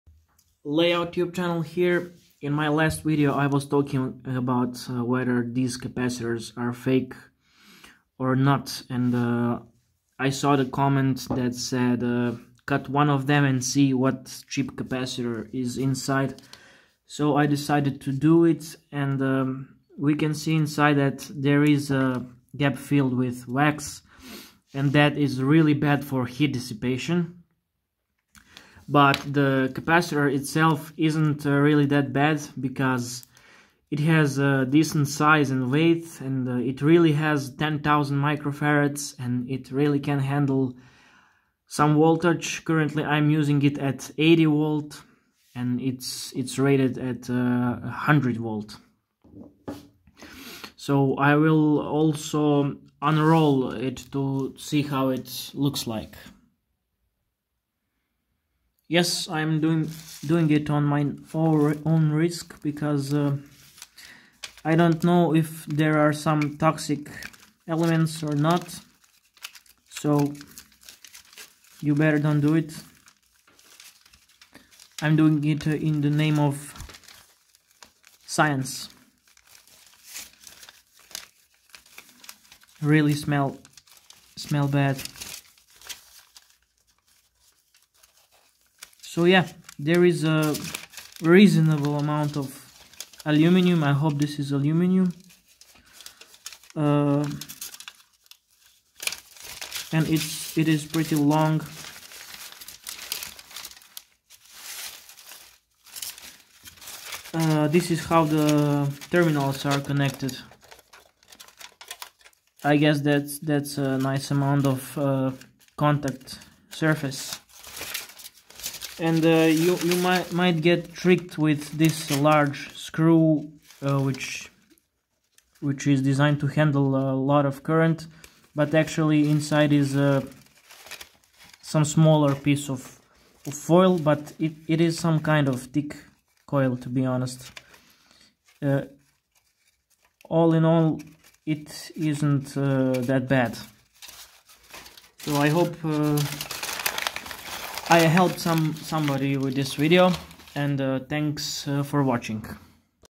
What's incide of fake ELNA 1000uF 100V electrolitic capacitor
internal construction of electrolitic capacitor from aliexpress